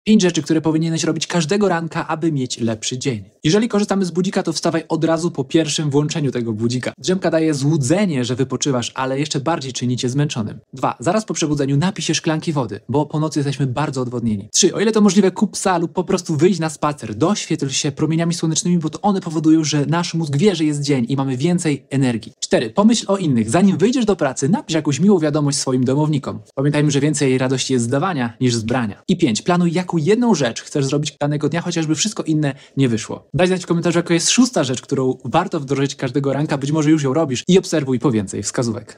Czy masz te 5 porannych nawyków?
Czy masz TE nawyki? Daj znać jaki jest Twój poranny nawyk, którego nie odpuszczasz! 😉